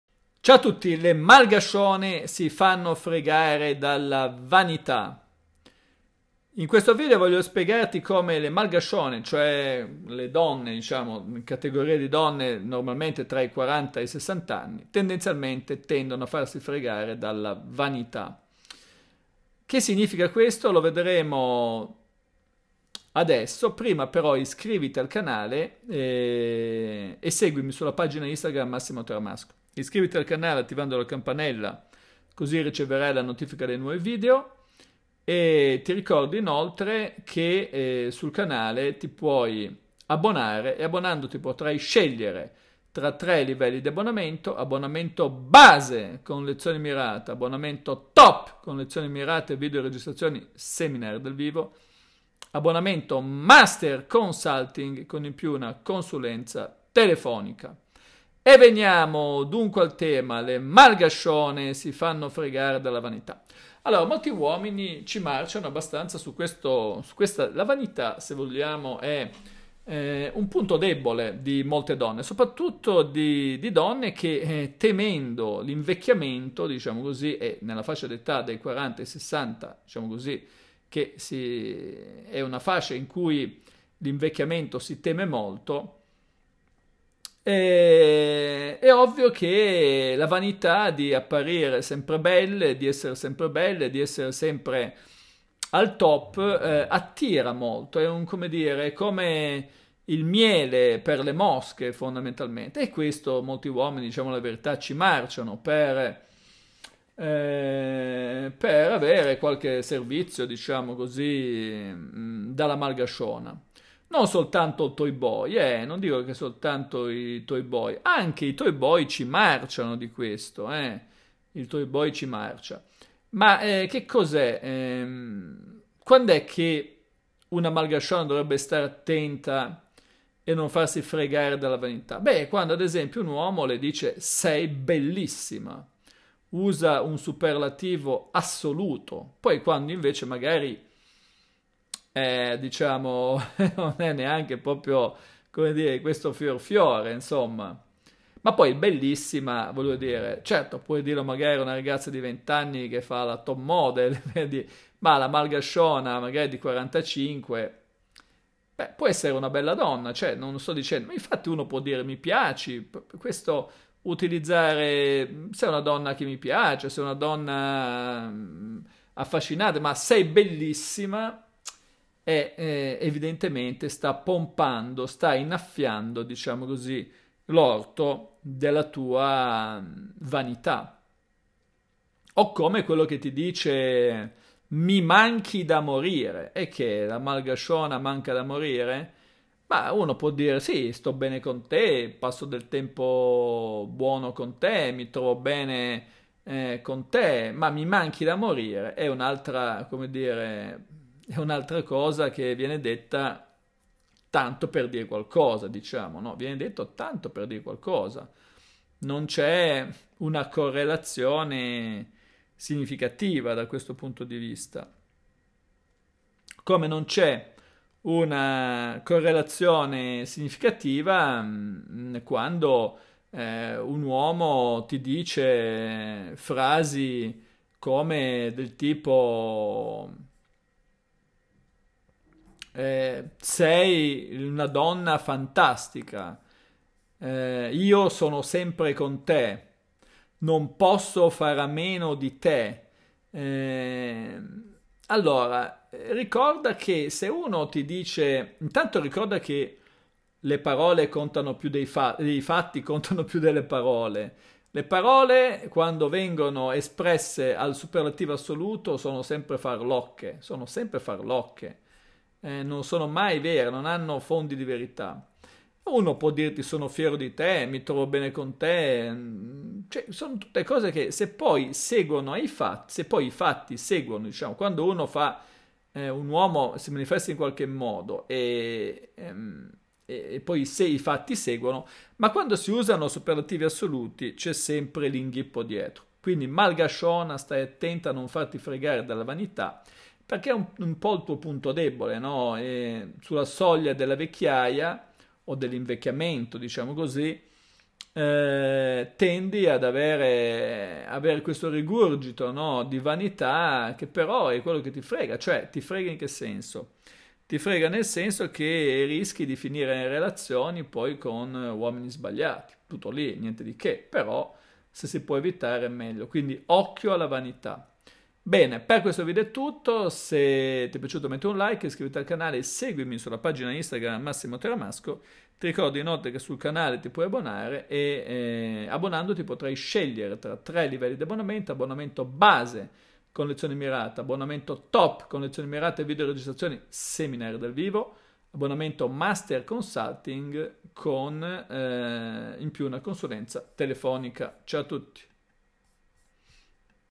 Le Malgascione si Fanno Fregare dalla Vanità (e i Toy Boys ci Marciano)
Le Malgascione si Fanno Fregare dalla Vanità (e i Toy Boy ci Marciano)
In questo video ti spiego perchè Le Malgascione si Fanno Fregare dalla Vanità (e i Toy Boy ci Marciano)